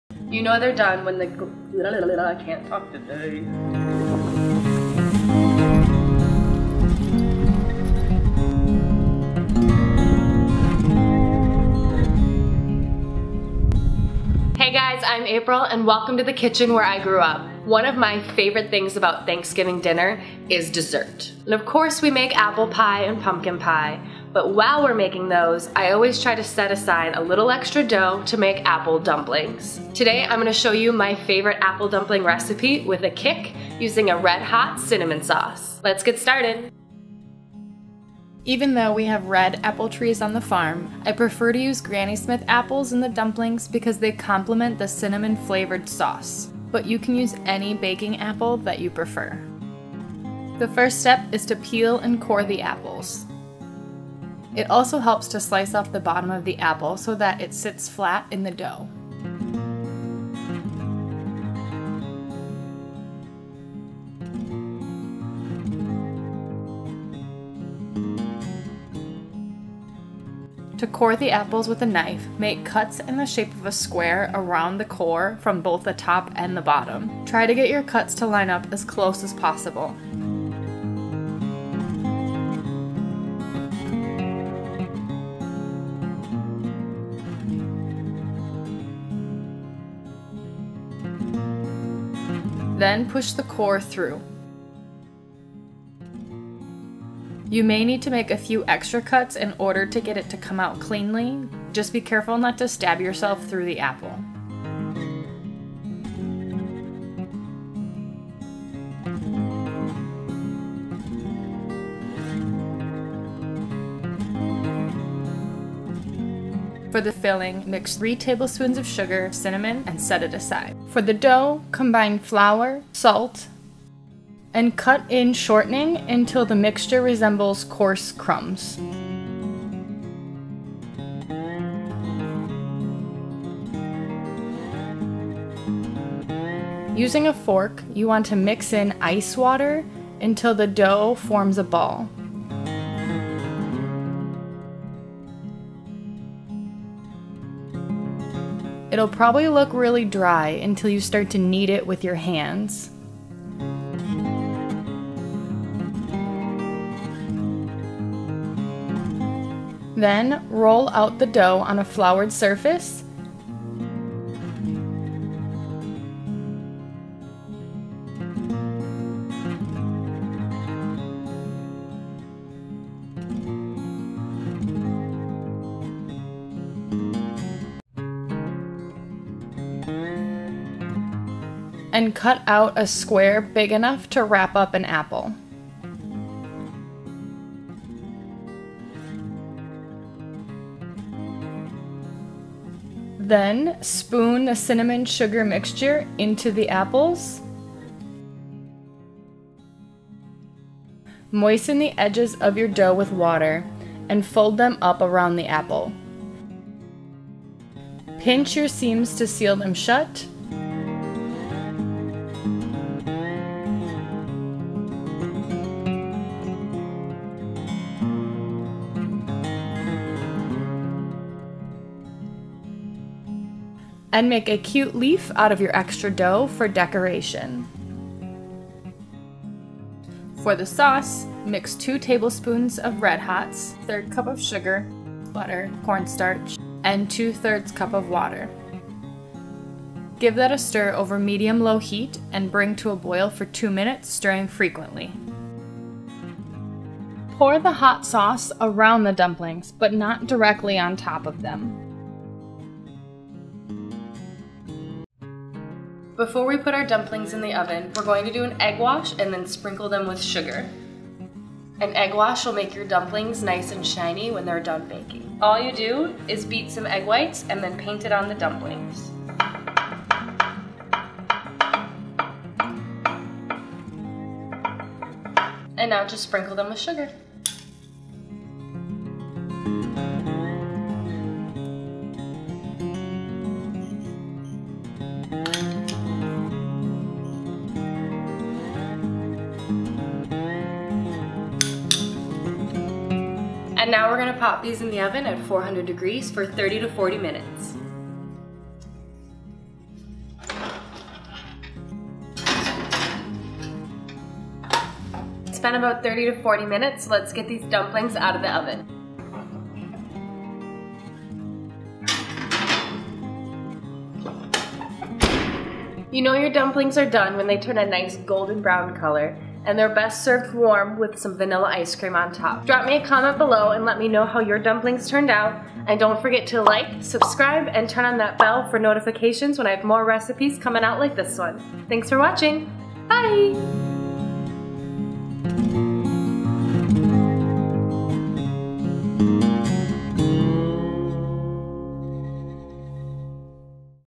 Red Hot Apple Dumplings
🍁🦃HAPPY THANKSGIVING! 🦃🍁 Dessert is my favorite part of Thanksgiving dinner .. here's a recipe for Red Hot Apple Dumplings that are just as delicious but not your typical apple pie.

Ingredients:

•  1 cup all-purpose flour
•  1/4 teaspoon salt
•  1/3 cup shortening
•  3 tablespoons ice water
•  2 medium baking apples
•  3 tablespoons sugar
•  1/2 teaspoon ground cinnamon

Sauce:

•  1/3 cup sugar
•  2 tablespoons Red Hots
•  1/2 teaspoon cornstarch
•  2/3 cup water
•  1 tablespoon butter